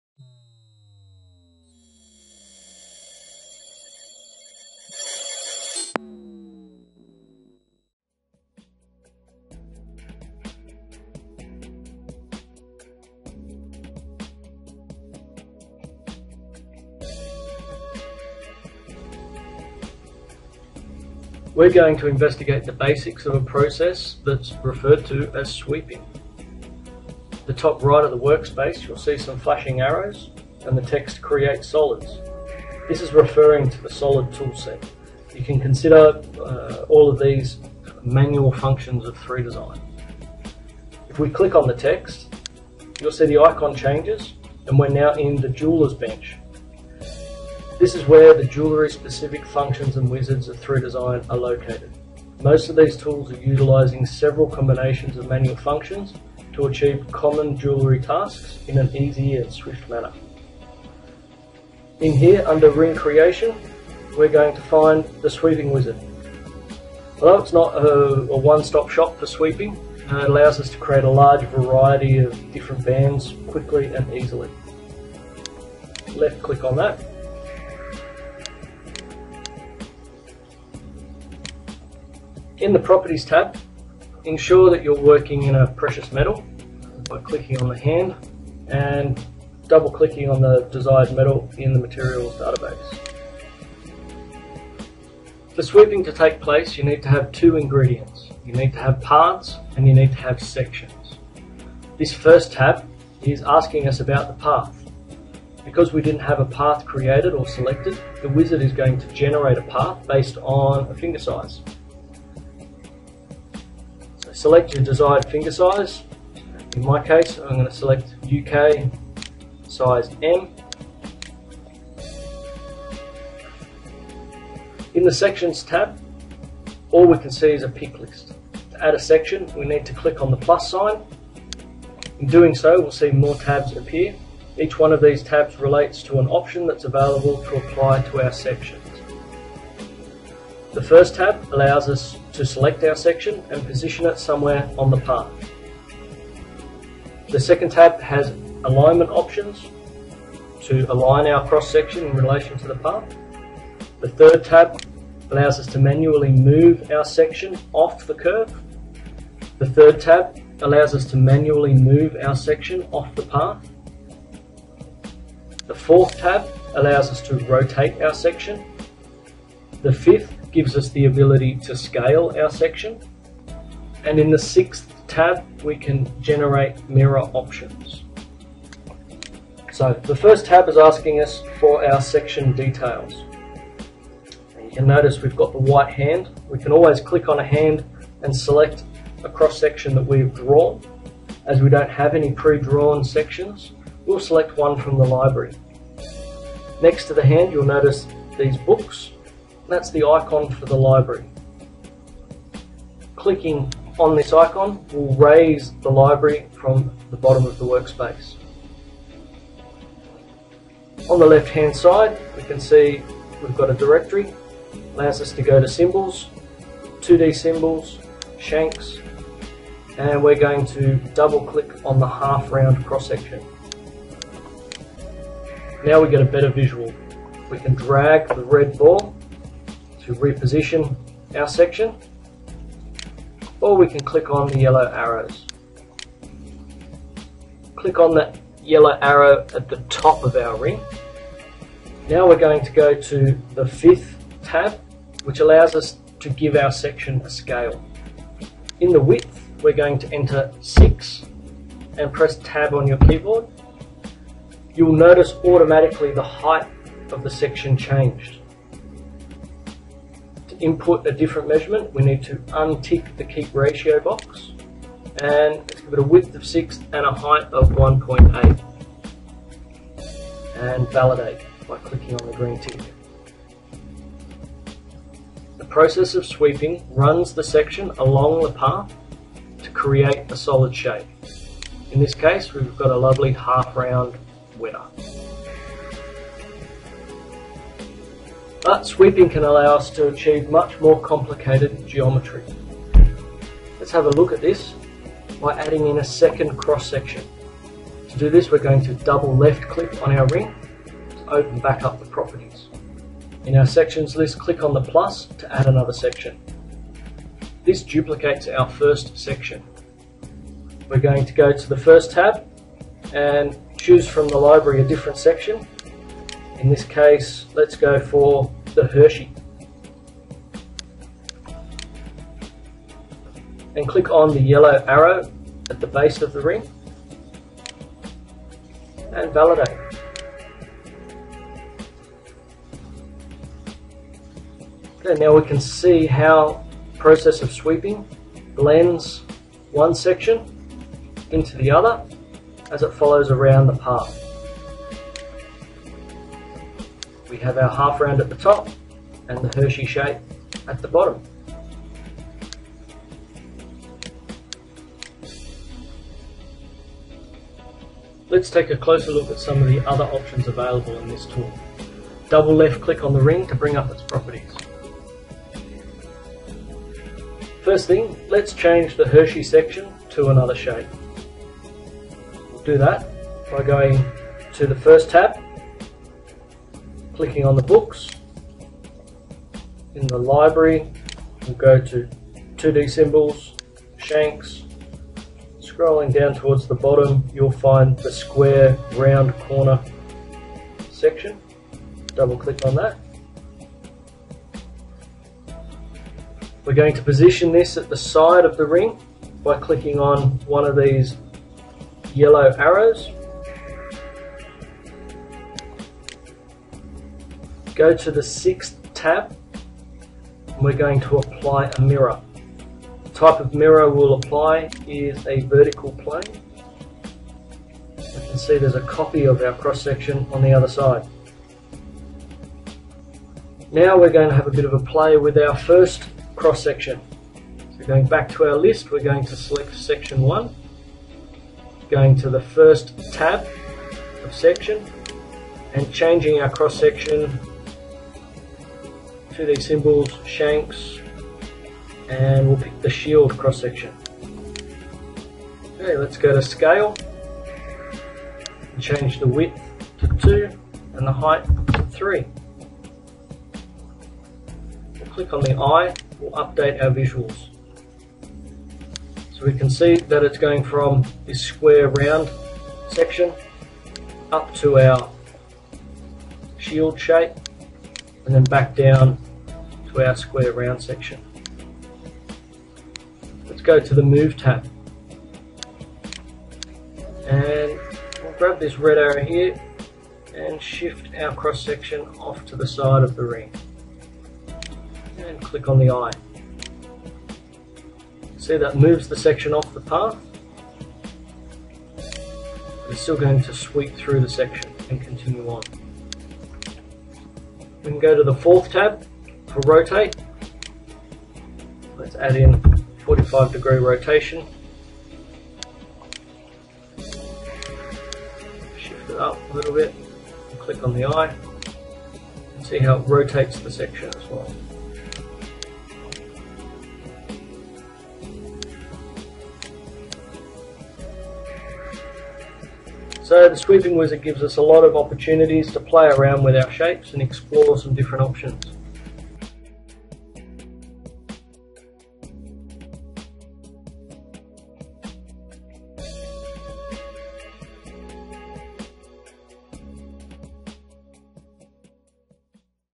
Lesson 03 Basic Sweeping
Sweeping is one of the most important functions of CAD software, learn how to achieve basic sweeping in 3Design.